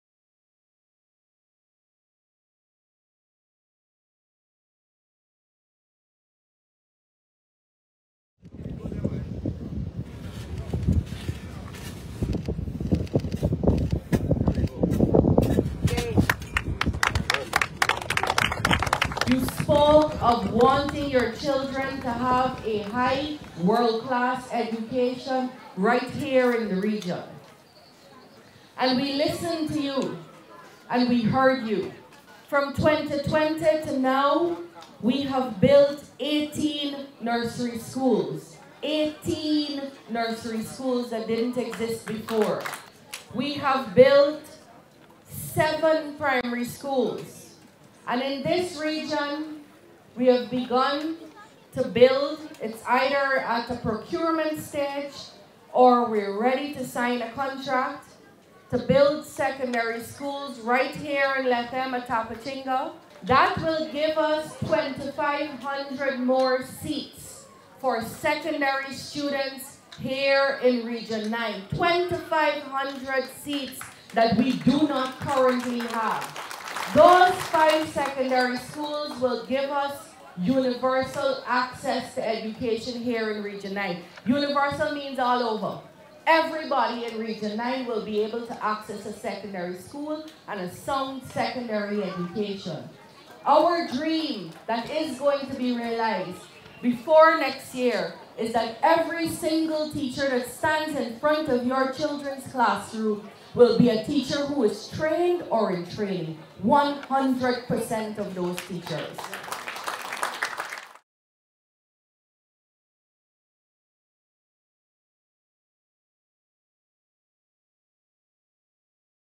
TABATINGA TO GET NEW MULTI MILLION DOLLAR SECONDARY SCHOOL
To address overcrowding and provide a comfortable environment for learning, a new secondary-level facility will be built at Tabatinga in Region Nine (Upper Takatu-Upper Essequibo). On Sunday, President Dr Irfaan Ali turned the sod to mark the beginning of the construction of the facility during a visit to the region. The school, which will be constructed to the tune of $182 million is expected to be completed within the next eighteen months. It will feature modern amenities which will integrate technology to make learning easier. Upon completion, it will house 800 students, significantly easing the current overcrowding at the St. Ignatius Secondary School.